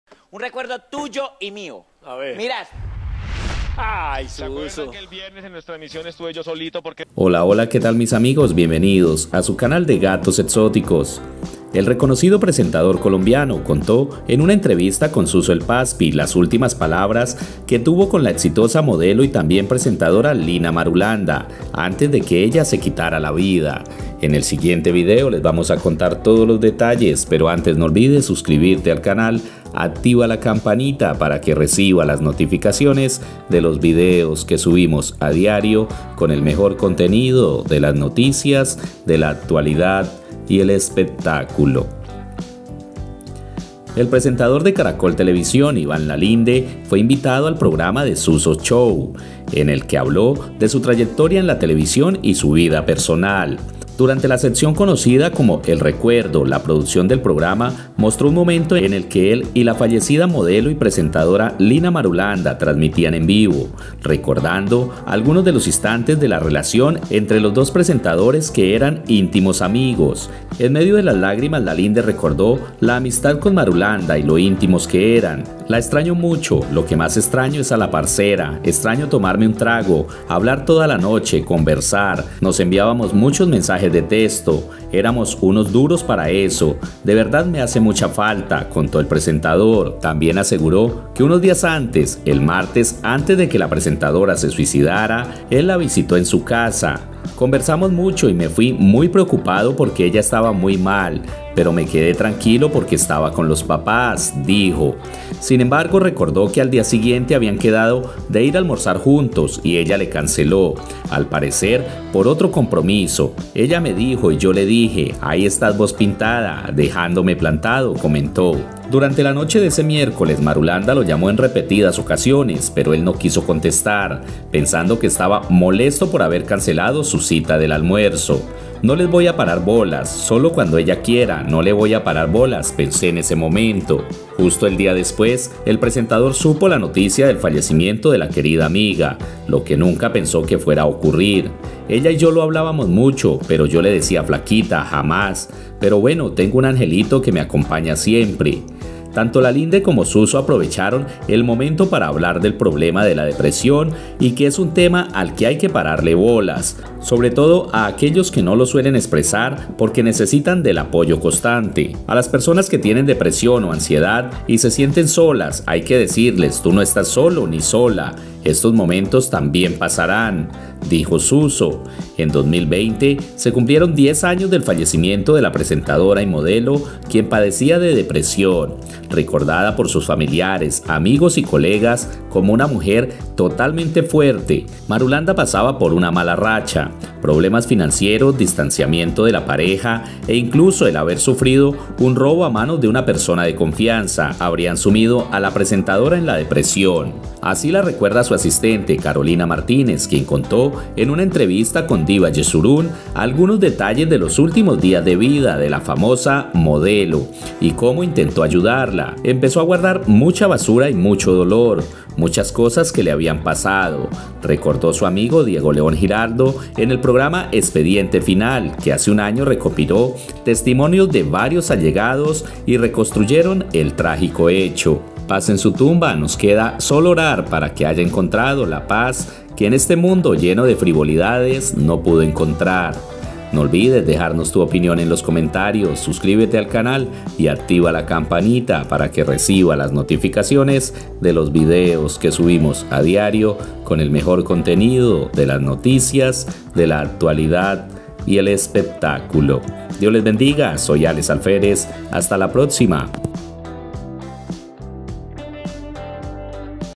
ME LLAMO ANTES DE QUITARSE LA VIDA Y NO LE CONTESTE (La triste historia del reconocido presentador)
El reconocido presentador conto que su amiga la tambien presentadora Lina Marulanda lo llamo antes de tomar la fatal decision pero el no quizo contestarle.